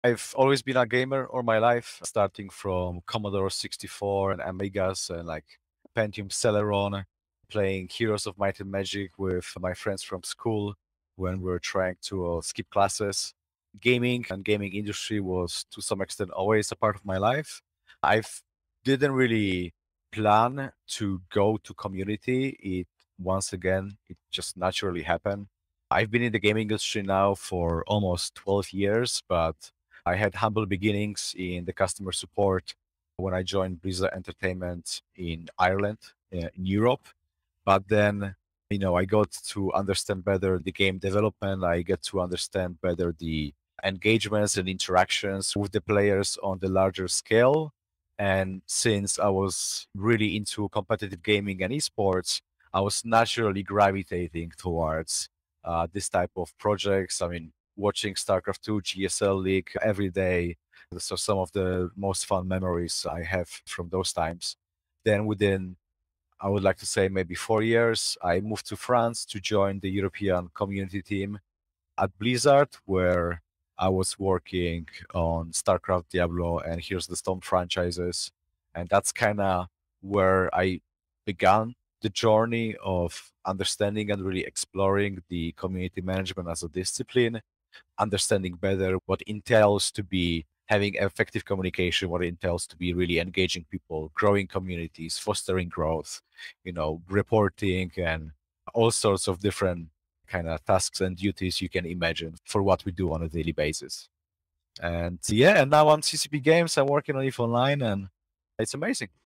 Kamil Wojtas: Career Journey of a Community Gaming Manager
From Commodore 64 to Heroes of Might and Magic to Blizzard Entertainment and now to CCP Games with EVE Online, learn about the community and gaming career journey of Kamil Wojtas out of Iceland.

"I've always been a gamer all my life, starting from Commodore 64 and Omega and like Pentium Celeron. Playing Heroes of Might and Magic with my friends from school when we're trying to skip classes. Gaming and gaming industry was to some extent, always a part of my life. I didn't really plan to go to community, it once again, it just naturally happened. I've been in the gaming industry now for almost 12 years, but I had humble beginnings in the customer support, when I joined Blizzard Entertainment in Ireland and Europe. But then, you know, I got to understand better the game development, I get to understand better the engagements and interactions with the players on the larger scale.

And since I was really into competitive gaming and eSports, I was naturally gravitating towards these type of projects. Watching Global StarCraft II League every day. So some of the most fun memories I have from those times. Then within, I would like to say maybe four years, I moved to France to join the European community team at Blizzard where I was working on StarCraft Diablo and Heroes of the Storm franchises.

And that's kinda where I began the journey of understanding and really exploring the community management as a discipline. Understanding better what entails to be having effective communication. What entails to be really engaging people, growing communities, fostering growth, you know, reporting and, all sorts of different kind of tasks and duties you can imagine for what we do on a daily basis.

And yeah, now I'm CCP Games and I'm working on EVE Online and it's amazing."